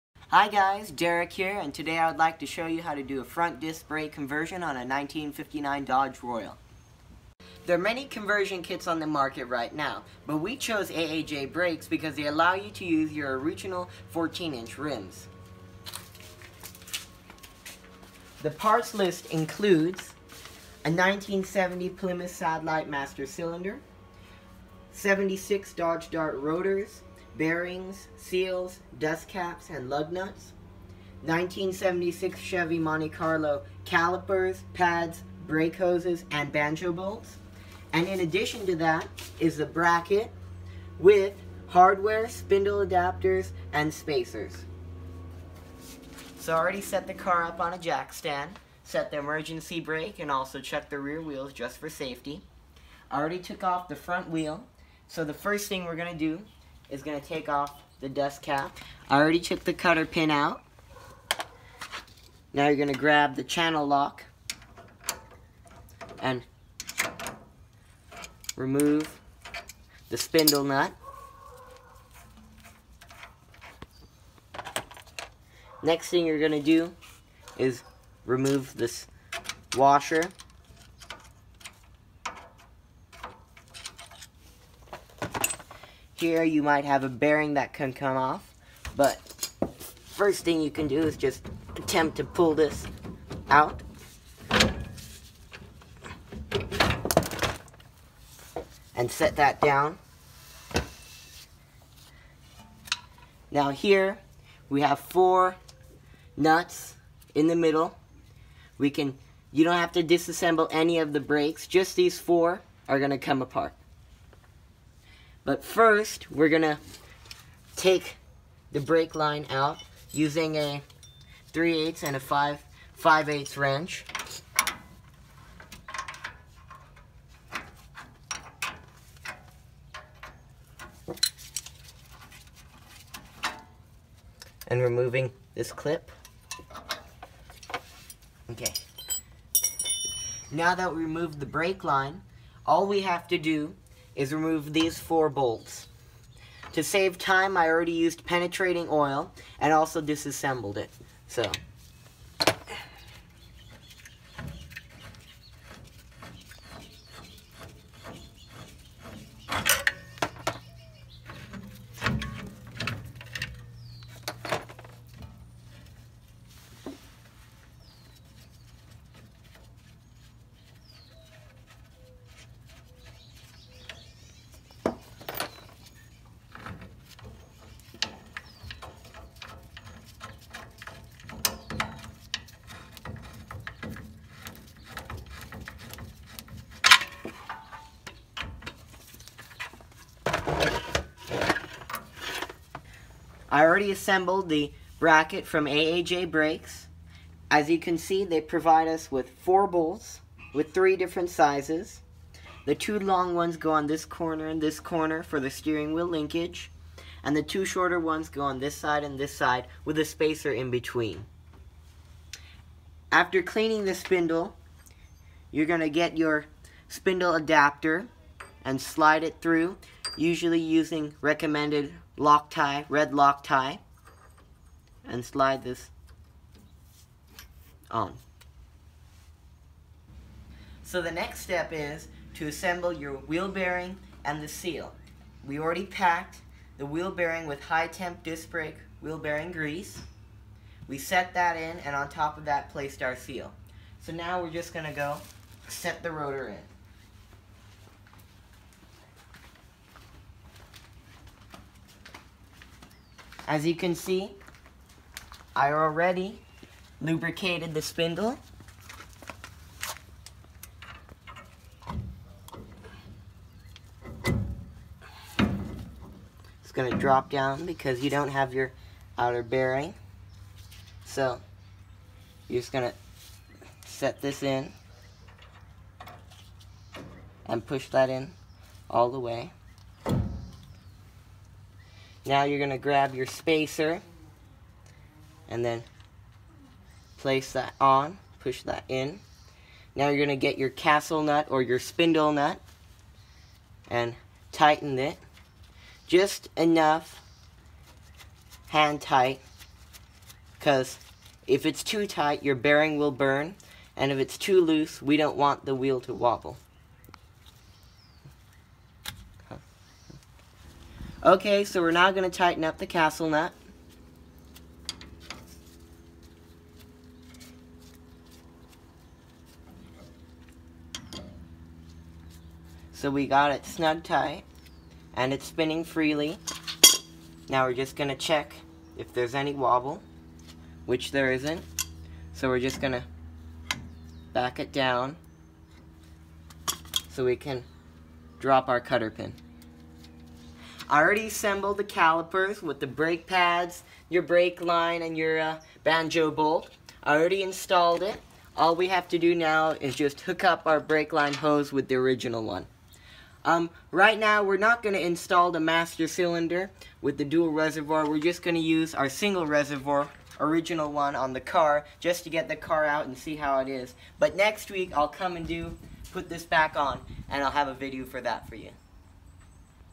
1959 Dodge Royal Front Disc Brake Conversion
1959 Dodge Royal Front Disc Brake Conversion.
361 Cid. V-8 with push button transmission.
Front disck brake conversion kit from AAJ Brakes located in Portland, Oregon.